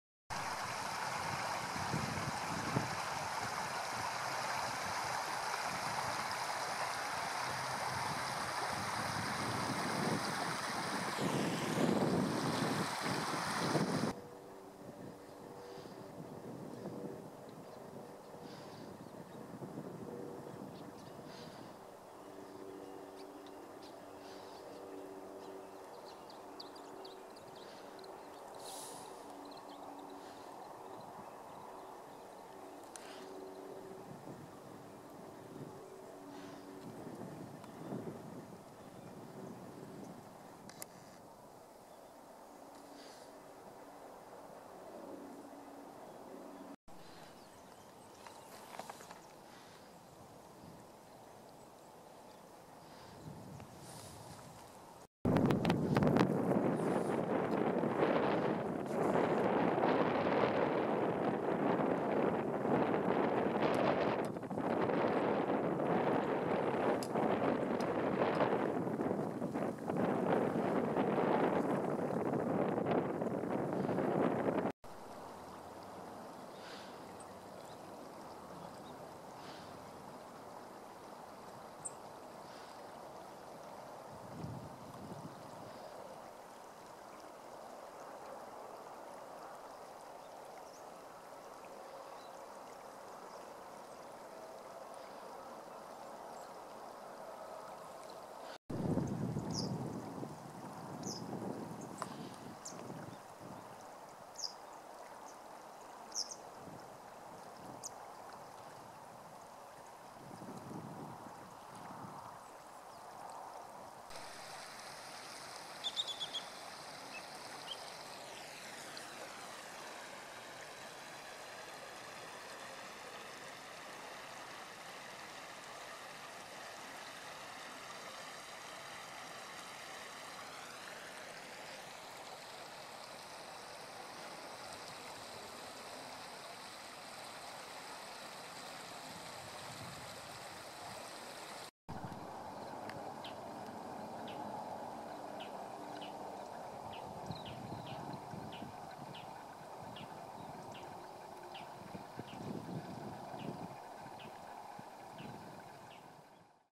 高麗川の鳥たち
２０１５年３月
イカルチドリ、キセキレイ、セグロセキレイ、イソシギ